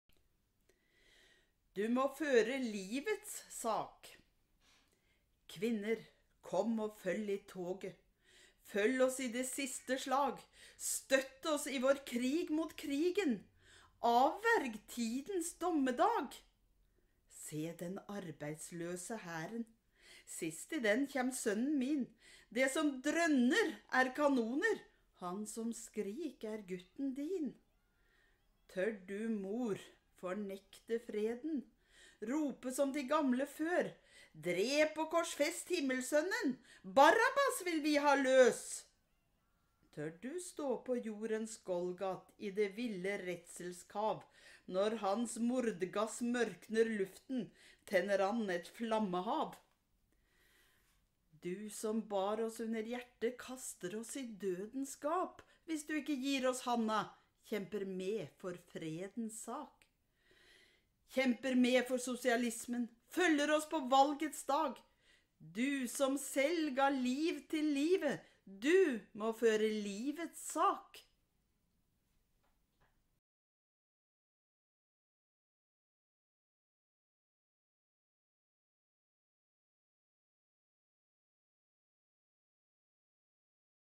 Du må føre livets sak
Kvinnekampdikt frå 1937, skrive av Ingeborg Refling Hagen, til samlinga Den Gamle Pioner. Skrive under inntrykk av kampane i Spania og som åtvaring mot den krigen som kom seinare.